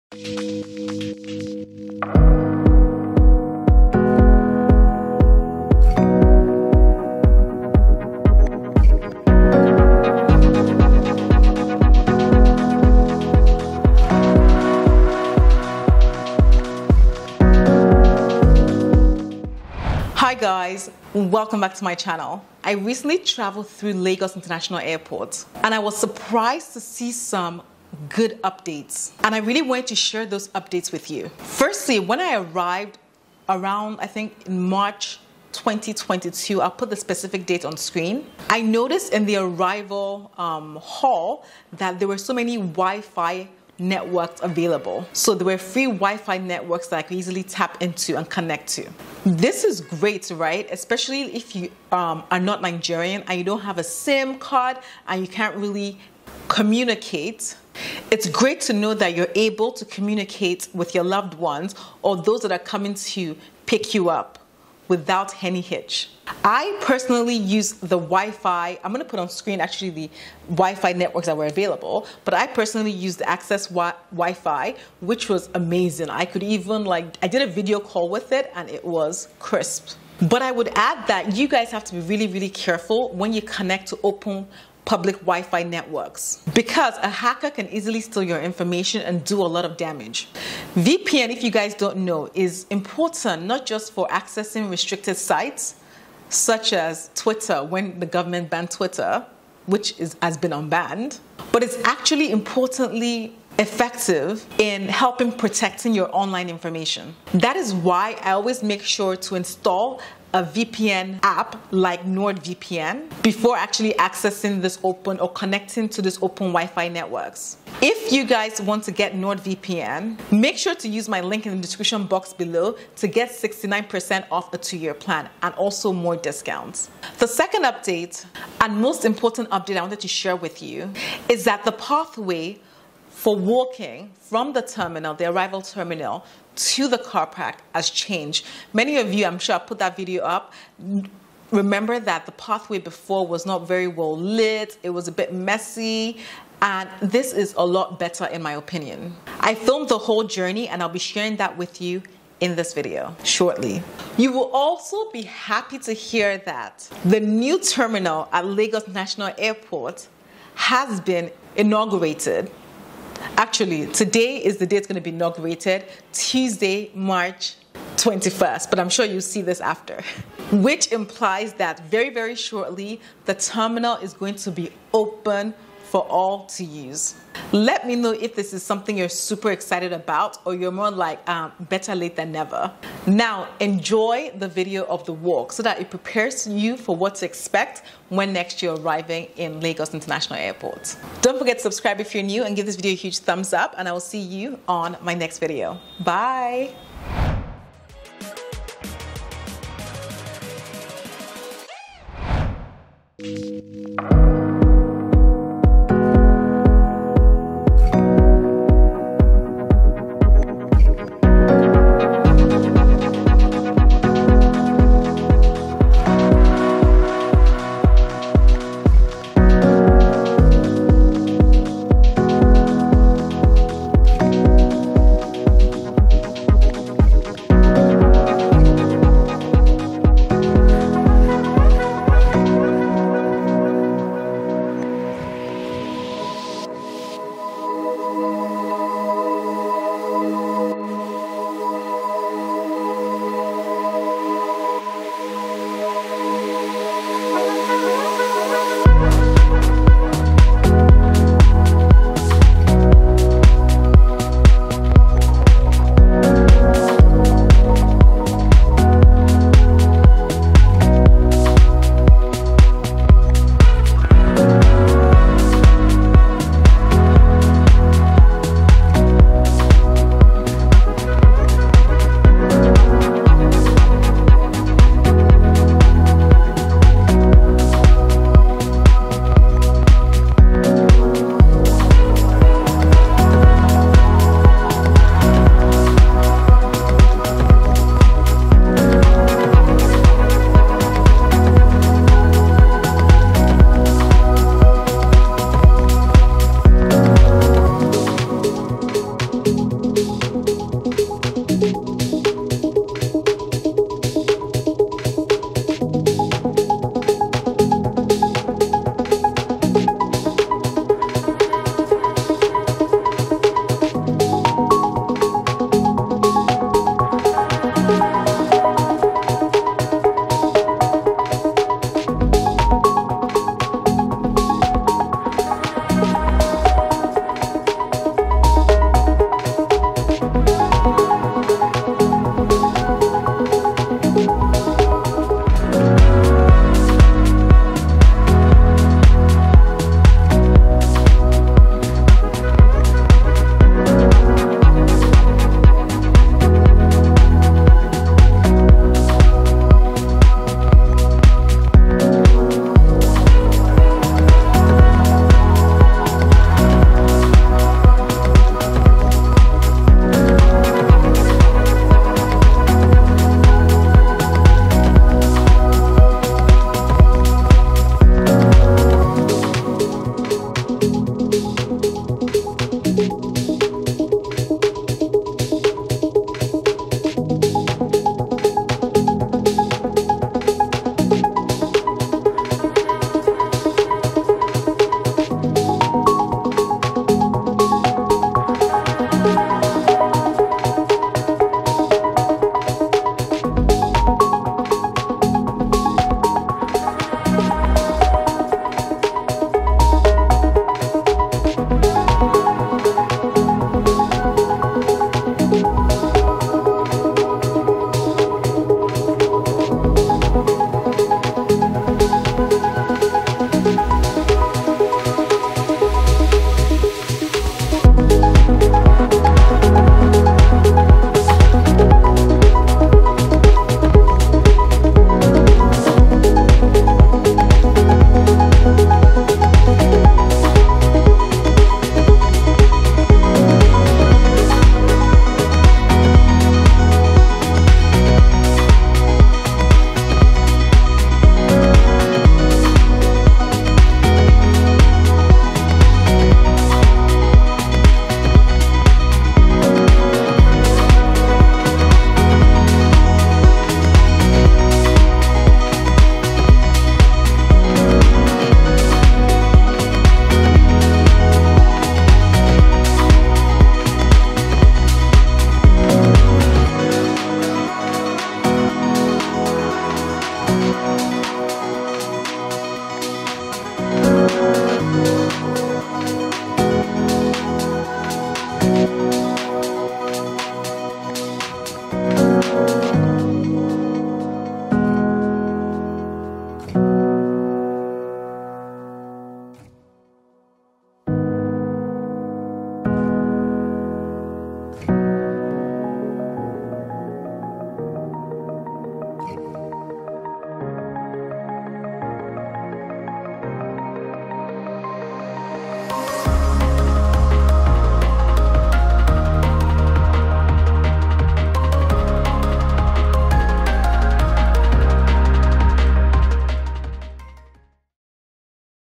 Lagos International Airport HAS CHANGED | NEW TERMINAL, FREE WIFI , NEW WALKWAY | Sassy Funke
Lagos International Airport HAS CHANGED | NEW TERMINAL, FREE WIFI , NEW WALKWAY |  Sassy Funke is a video where Sassy Funke share some NEW EXCITING UPDATES at Lagos International Airport from the new international airport terminal that was commissioned on March 22nd by our president to the new walkway created for travelers.

Check out some VALUABLE resources for travel to/out of Nigeria

Chapter
0:00 Lagos Airport New Terminal
0:35 FREE WIFI Access
3:24 NEW WALKWAY (walk with me)